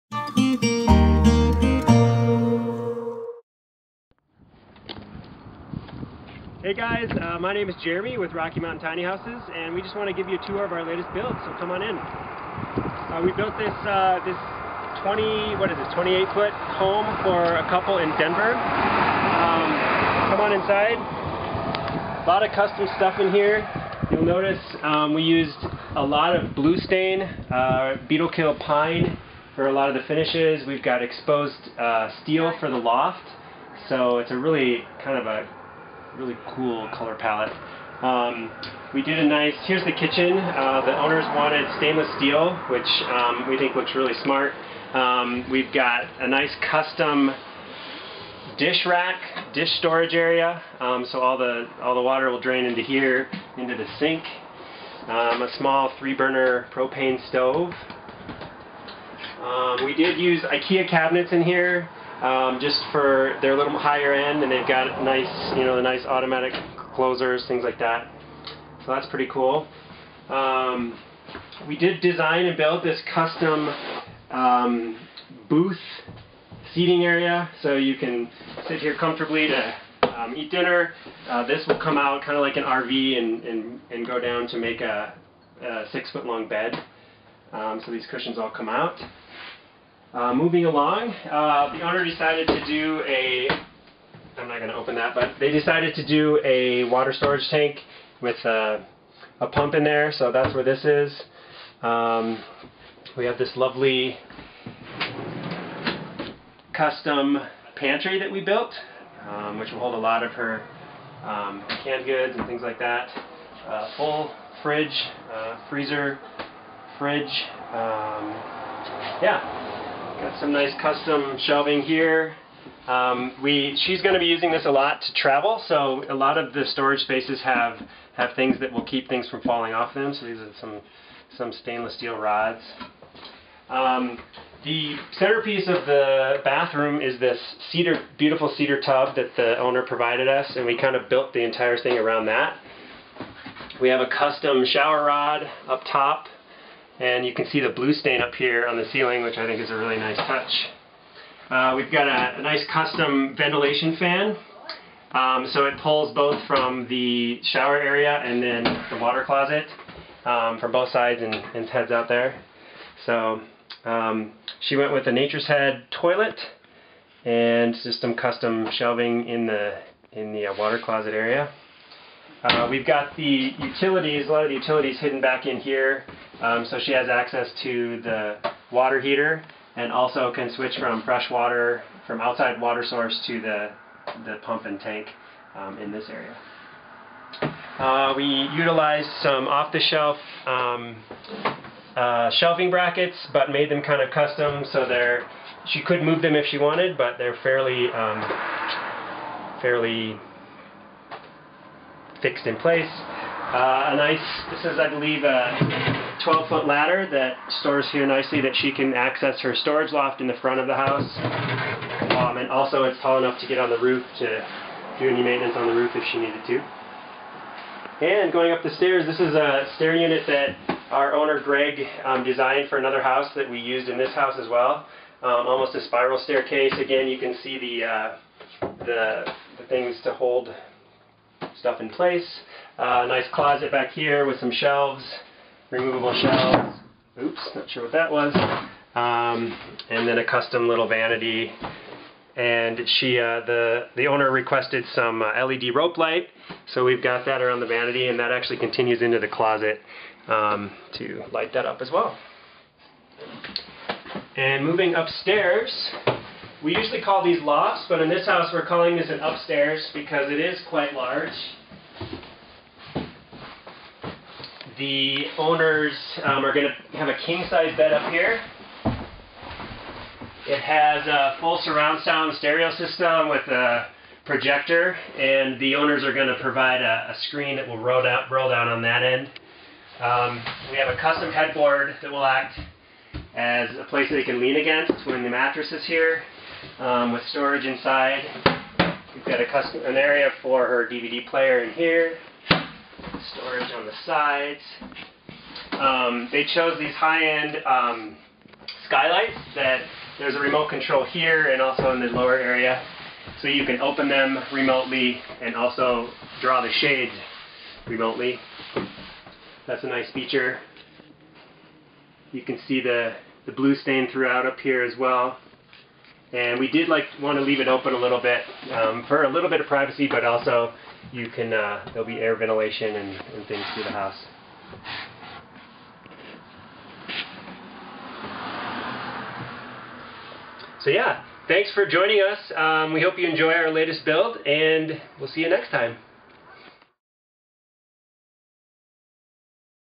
The Ol' Berthoud Blue by Rocky Mountain Tiny Houses
Alden was very much wanting to build and own a tiny house. After quite a bit of difficulty building one for herself, he commissioned Rocky Mountain Tiny Houses in Durango, Colorado. Here's a quick blurb from Rocky Mountain Tiny Houses who was kind enough to provide this video tour of their build to us.

"The kitchen features modern Ikea cabinets with a custom stainless countertop, including a large sink and triple burner cooktop. A custom built dish rack not only stores dishes but lets them drip dry into the sink. Opposite the kitchen is a built in dining table that converts to a daybed for lounging or overnight guests. The bottom panels pull out for additional storage below. Behind the dining set is a small counter that can be used for prep space or a small office area. Behind this is a compartment housing a 60 gallon freshwater tank for instances when they aren’t hooked up to domestic water since this house is intended to be on the road a lot over the coming years. The walnut top comes off for access and can used for more prep space when access is not needed."